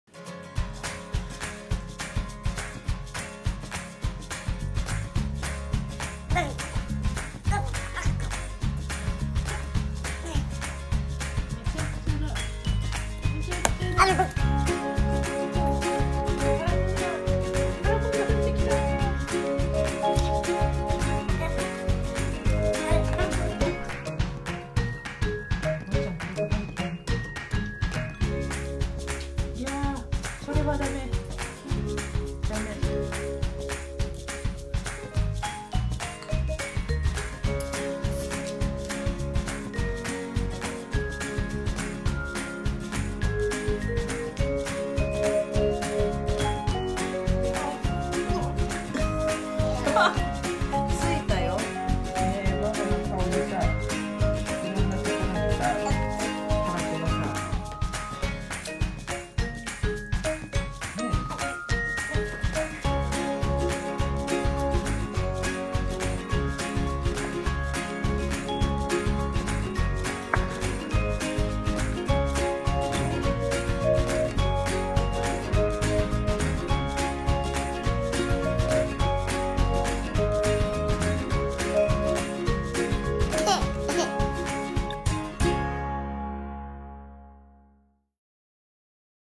手裏剣ごはん。Ninja dinner
はじめてのたらこは手に、腕に、顔に、髪の毛に、ママの顔に、床に。。。本人は結構楽しかったらしい🤣

Our son enjoyed his first time eating pasta with cod roe. He loved the taste but I guess he doesn’t like things stuck on his hands...!

▼この食後のウエハダカ/What happened after this dinner